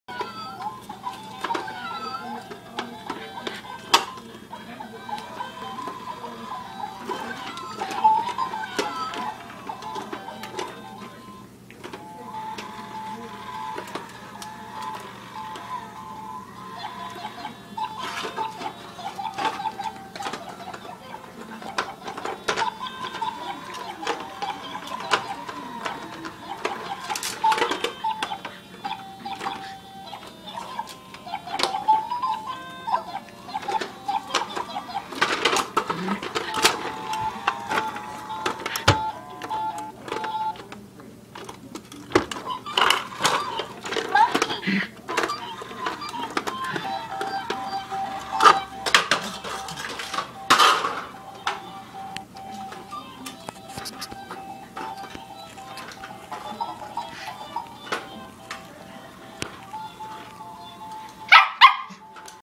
Sheltie Puppy v Talking Baby Toy
My puppy Heidi when we first got her and she fell in love with this talking baby ball.